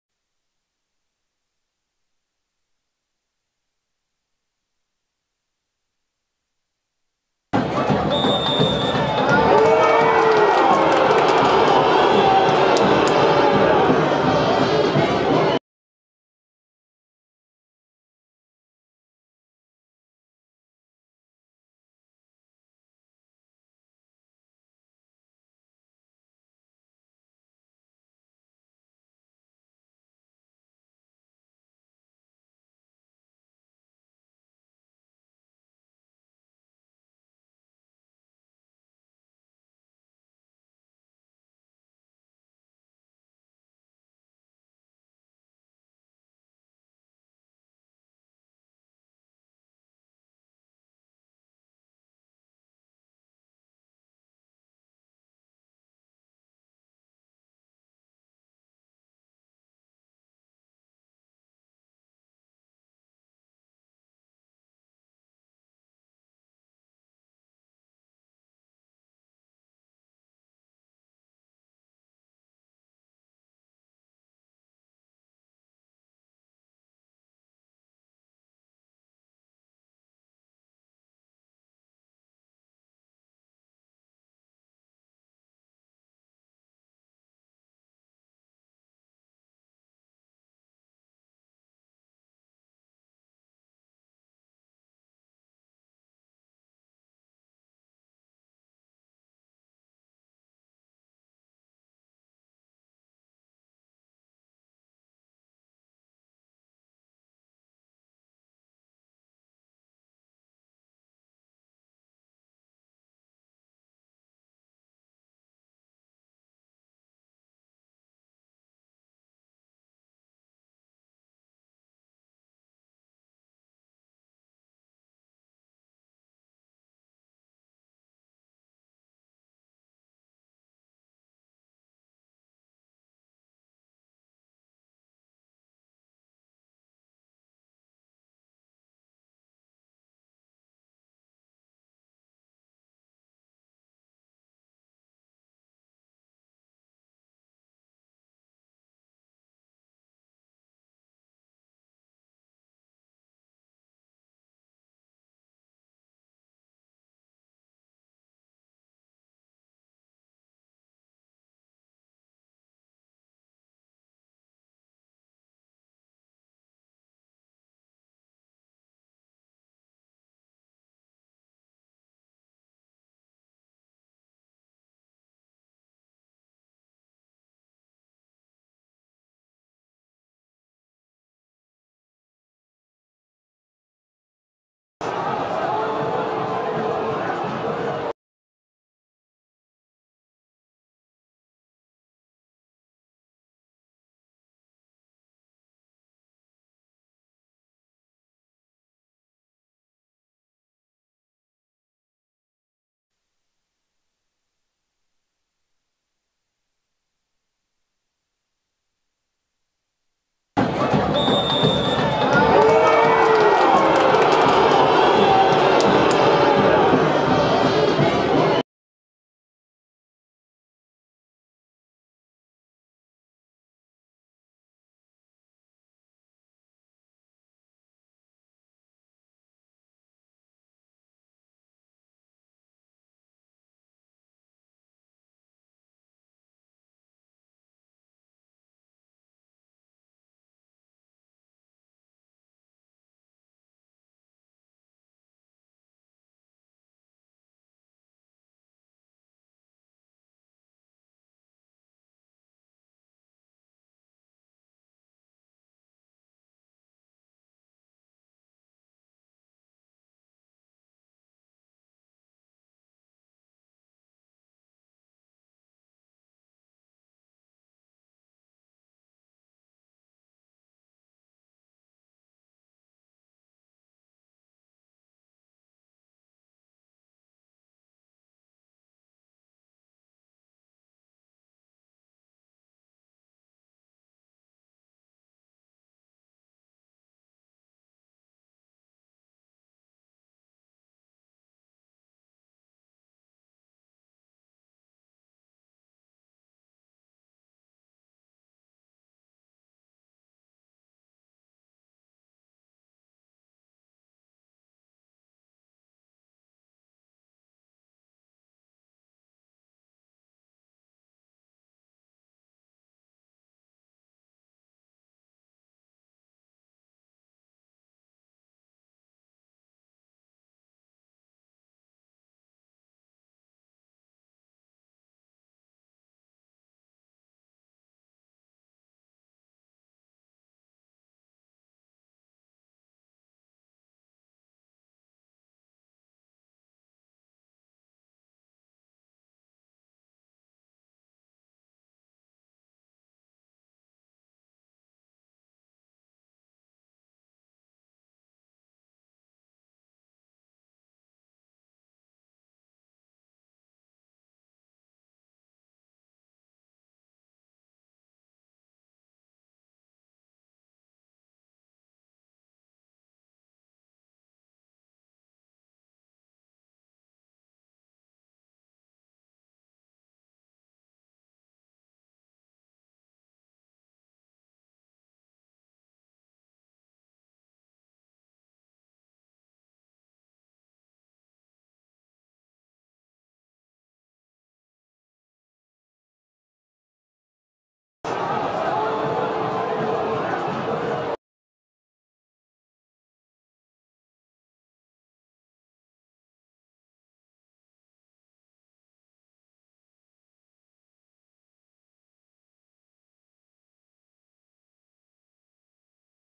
COMPACTO BRASIL 0 x 1 FORTALEZA - Série B.
Confira os melhores momentos entre BRASIL 0 X 1 FORTALEZA.